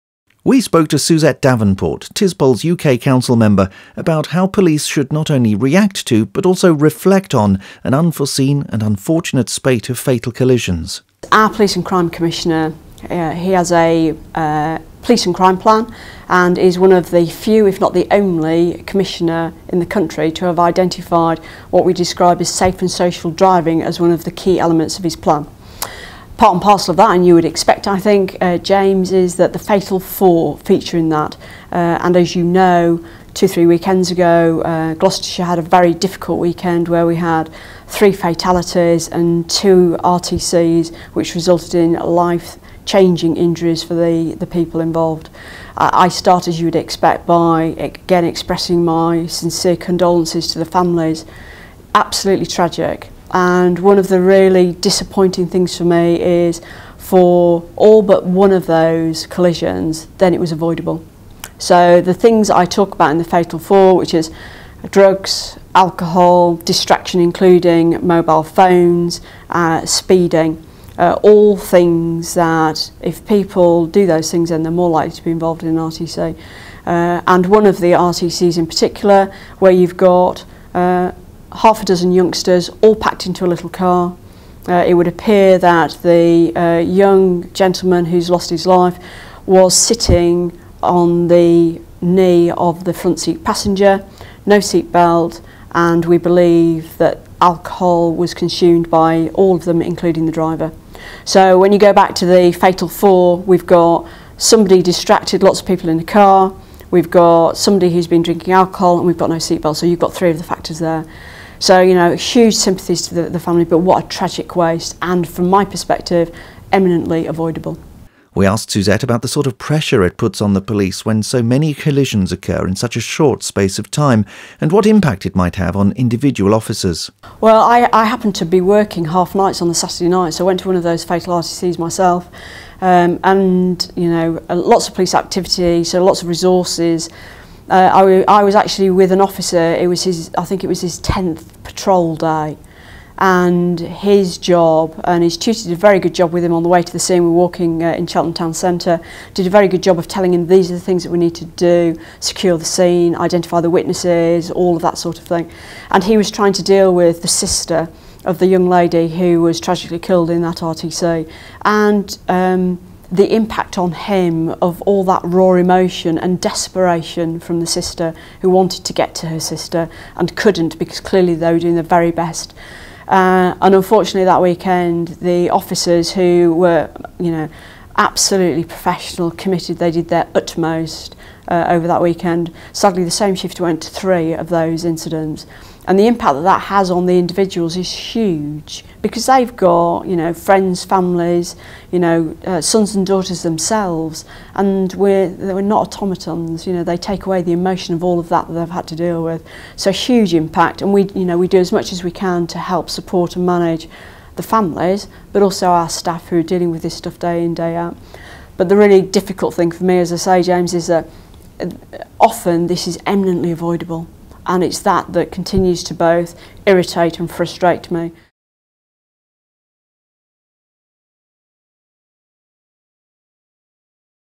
TISPOL video on road safety
This new video from Pan-European police body TISPOL details the effects of a series of tragic road crashes in the UK. TISPOL's UK council member Suzette Davenport gives a personal account of her experience as attending officer at recent road crashes in the UK.